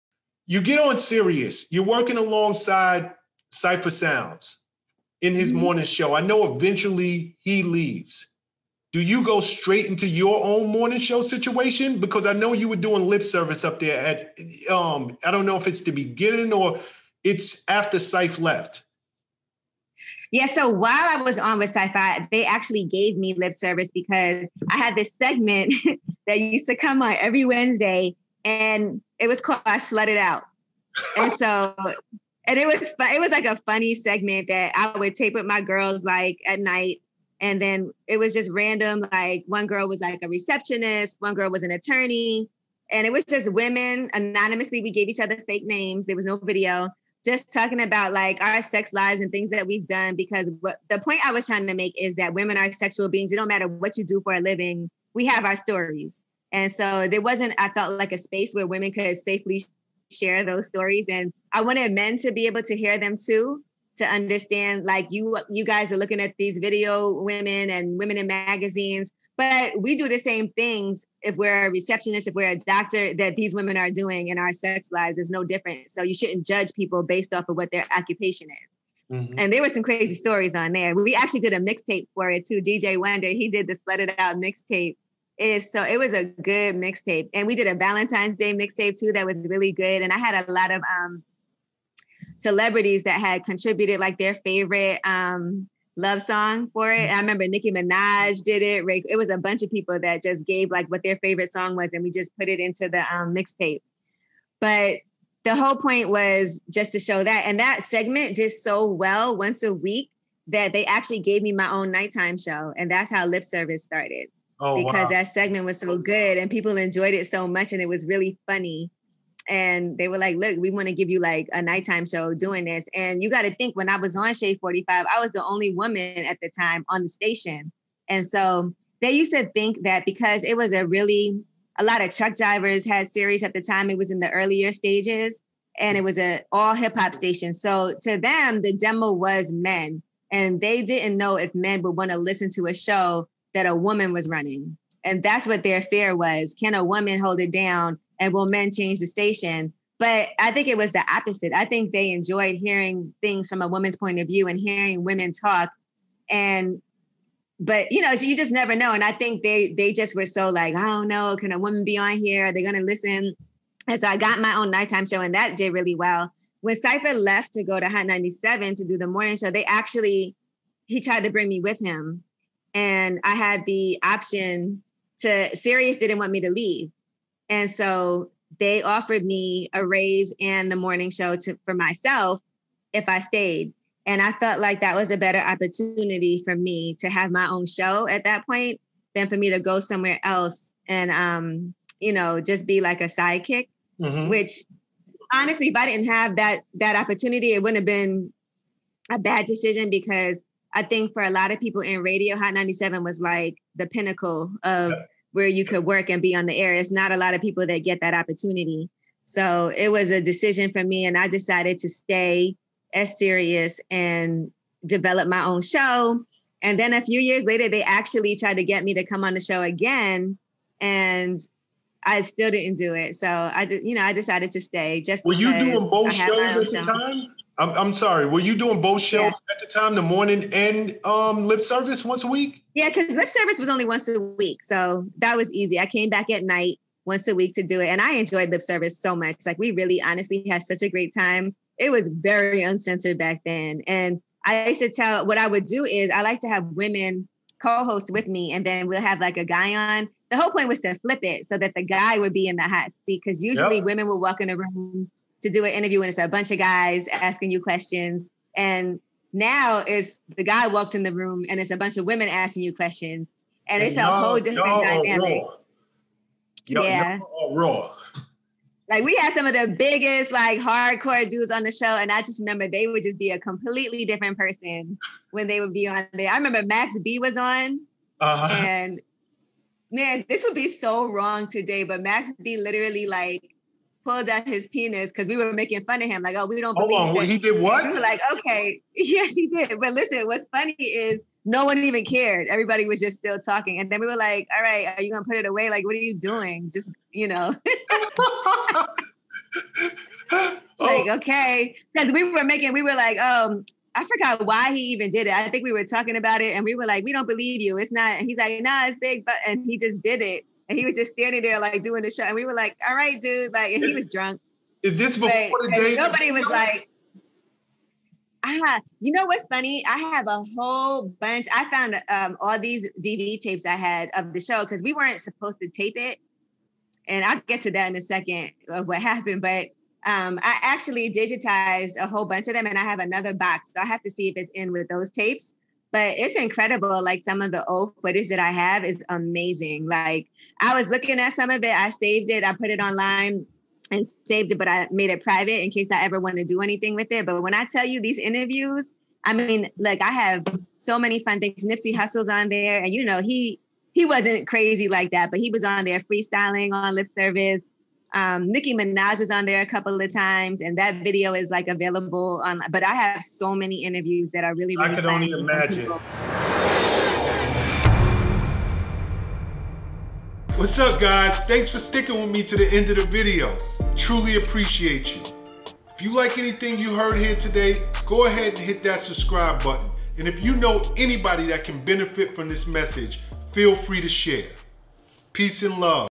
Angela Yee - You Must Be Willing To Pass On Some Opportunities. Stay Focused on Your Goal!
Angela Yee landed The Breakfast Club by staying focused on her goals. She had several enticing opportunities in front of her, but she stayed patient and faithful that the right opportunity would come through and it did!